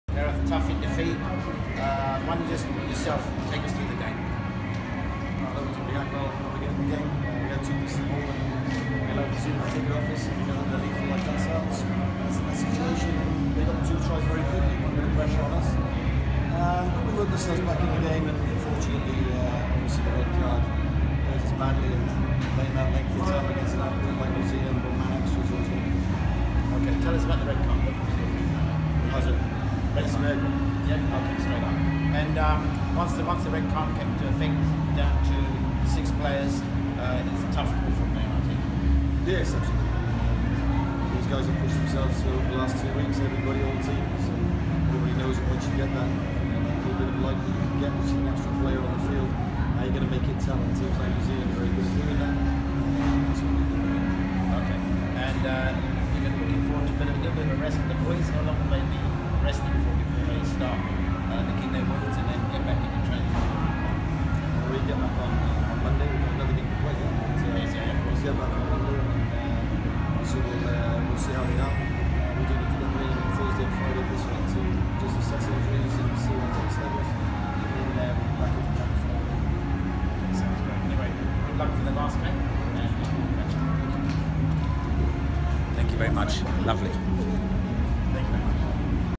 Fiji 7s coach Gareth Baber post-NZ match interview
Fiji 7s coach Gareth Baber speaks with Fiji's Amrit Bhai Amrit after the match they lost to New Zealand at the Sydney 7s. Video by AJAY BHAI AMRIT